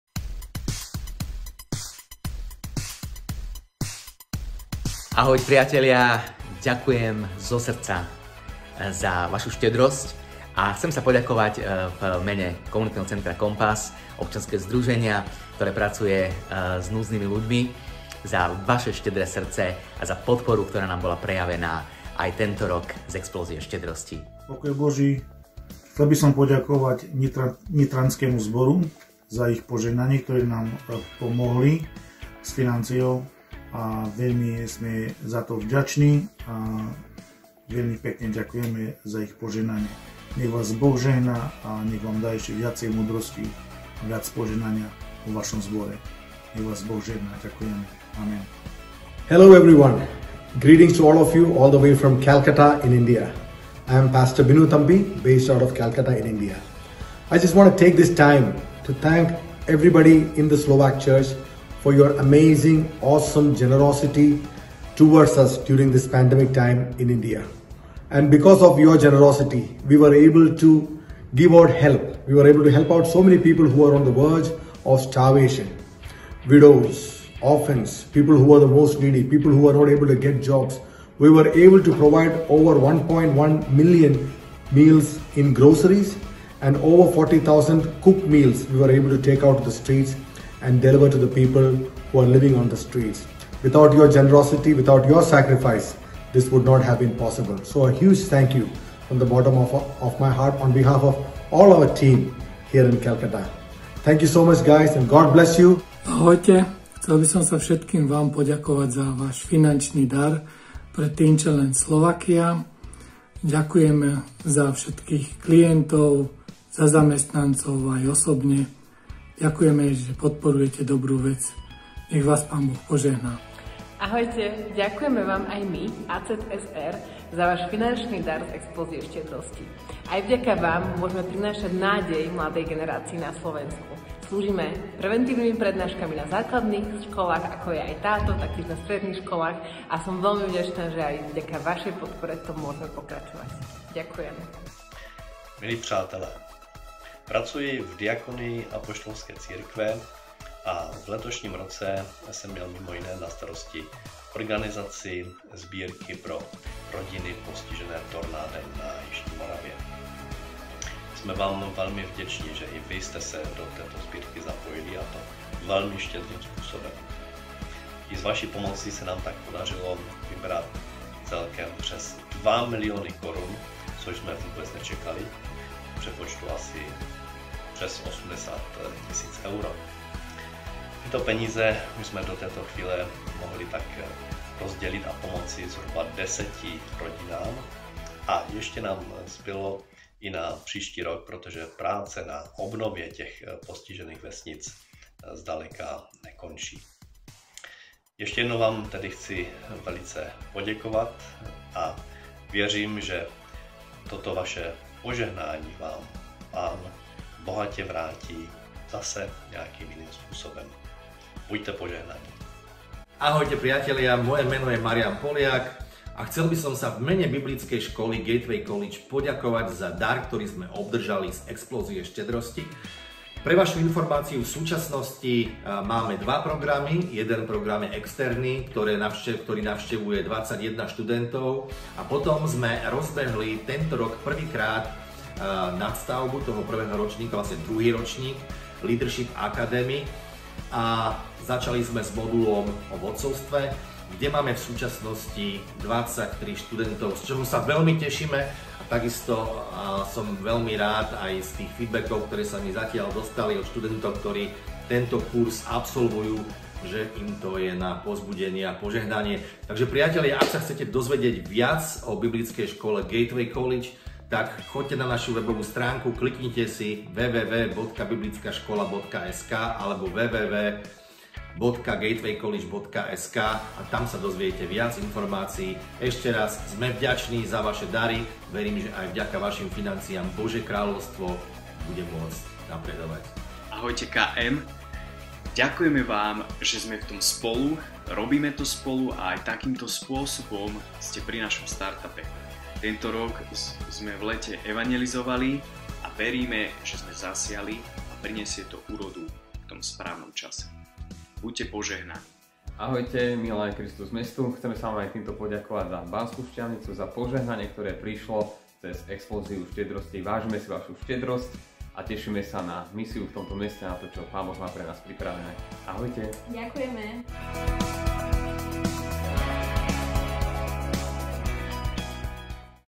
Dary z Explózie štedrosti 2021 (plná verzia)
Ďakujeme, že ste sa pridali! Stali ste sa súčasťou investície do Božieho Kráľovstva, ktorá má dopad od jednotlivca až po globálne služby. Veríme v dávanie, zasievanie a žehnanie! To, čo nám Boh dal chceme investovať ďalej do Jeho Kráľovstva.
Môžete sa zapojiť opäť v kampani Explózia štedrosti 2022.